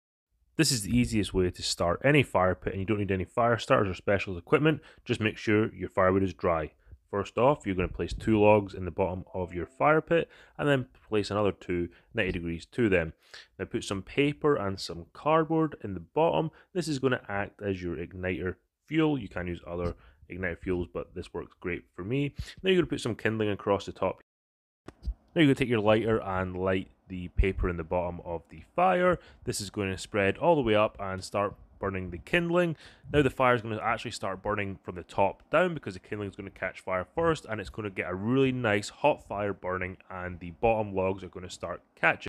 How to start a Fire Pit the Easy way
How to start a fire pit using the top down fire method.